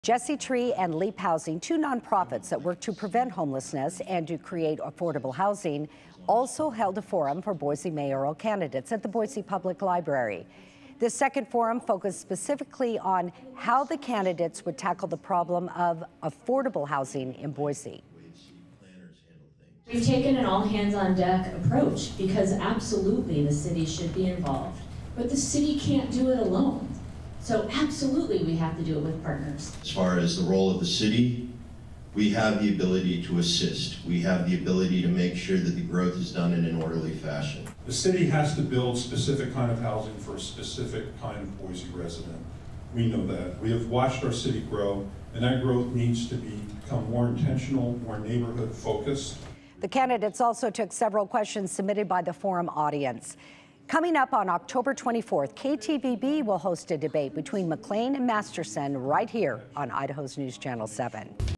Boise mayoral candidates speak on affordable housing
The forum was focused on how mayoral candidates would tackle the problem of affordable housing in Boise.